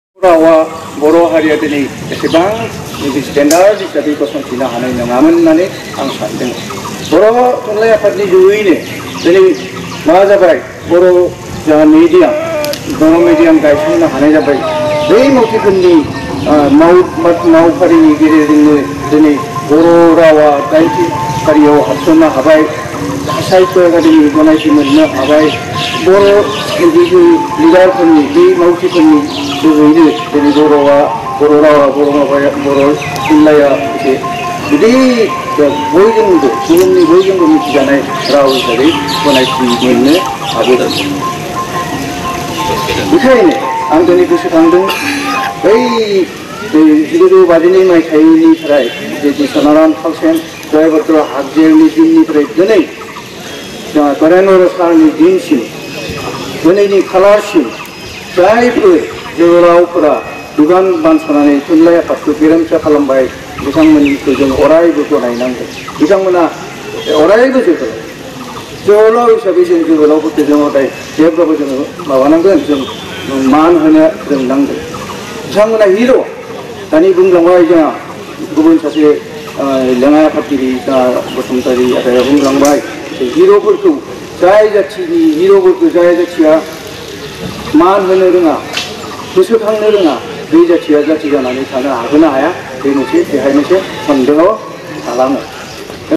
आगु थुनलाइ सिबिआरिफोरनि नाजानायाव बर'थुनलाइ आफादा दिनै बेसेबांबा गोजौ थाखोआव मोनफैगोन हाबाय: सितानाथ
Literary speech